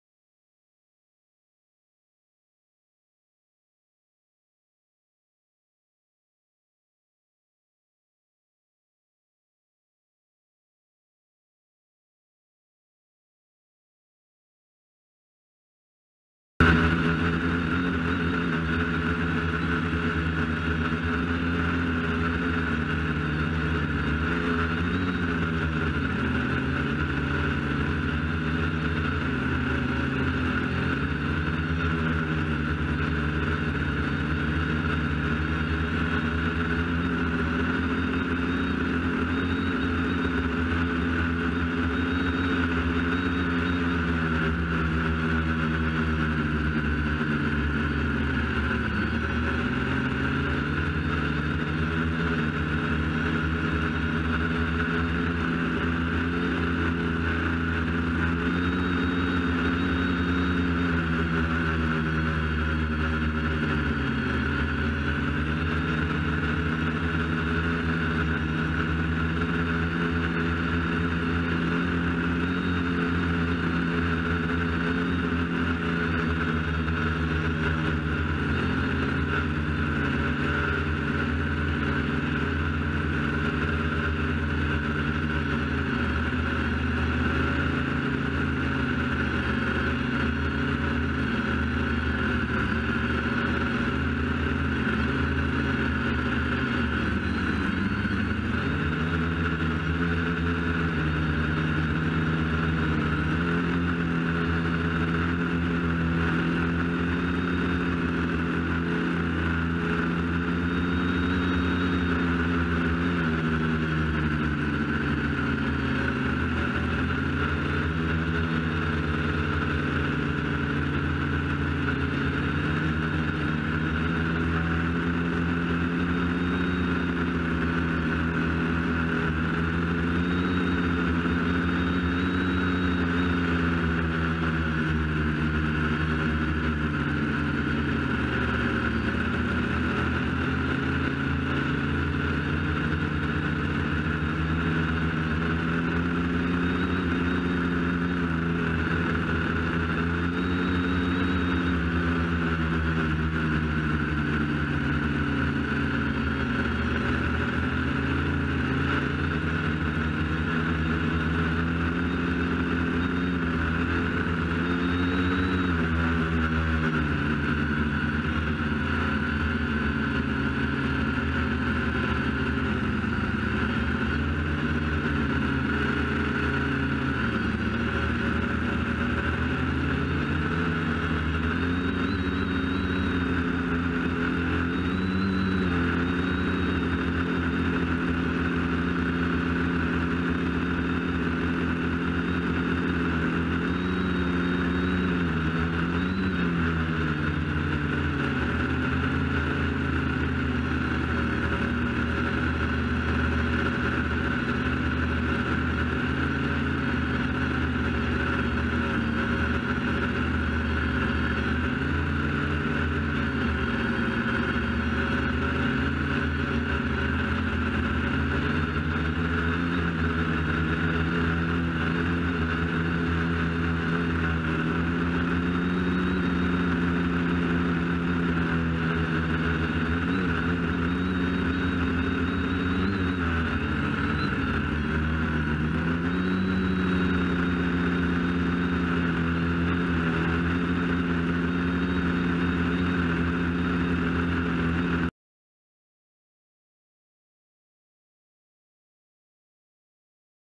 Nieuwvliet from the sky 2016-08-16
An overview from the sky of the Dutch village Nieuwvliet  in the Dutch province of Zeeland on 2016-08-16...